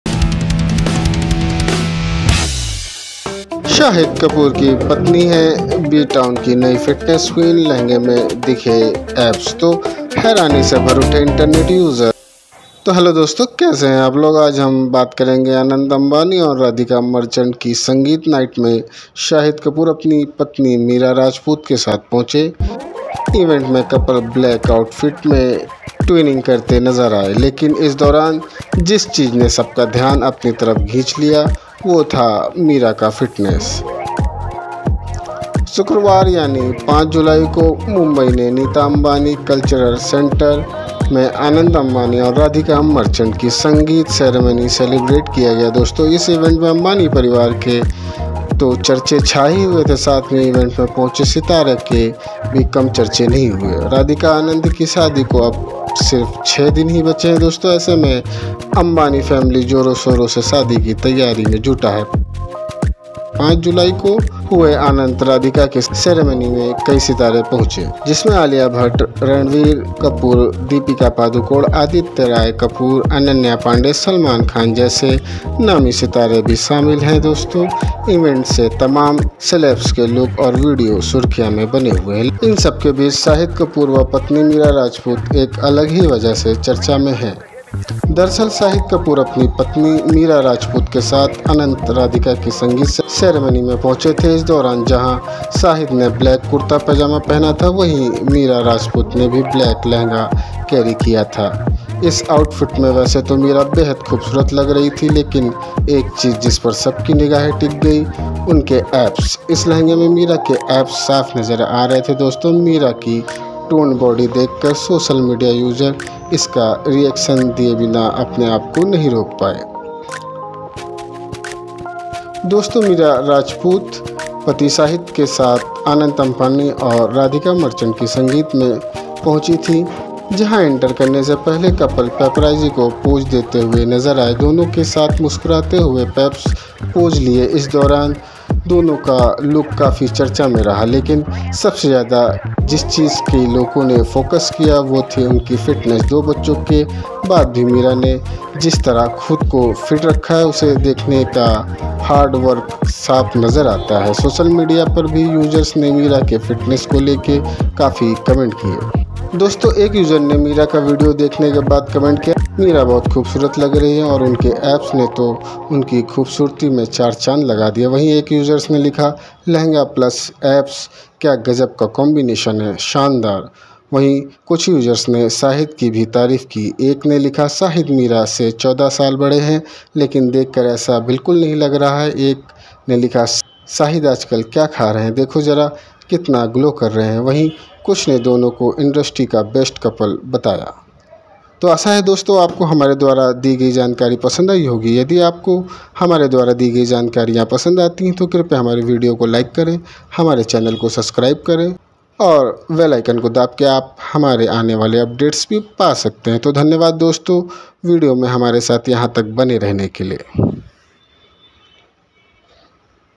Shahid Kapoor's Wife: The New Fitness Queen of B-Town! Abs in Lehenga Surprise Internet Users
Get ready to be amazed as Shahid Kapoor's wife, Mira Rajput, takes the internet by storm with her incredible fitness transformation! In this video, we reveal how Mira has become the new fitness queen of Bollywood, showcasing her toned abs even while wearing a traditional lehenga. Watch as we dive into her fitness journey, the routines she follows, and the reactions from fans and celebrities alike. This is one transformation you don't want to miss!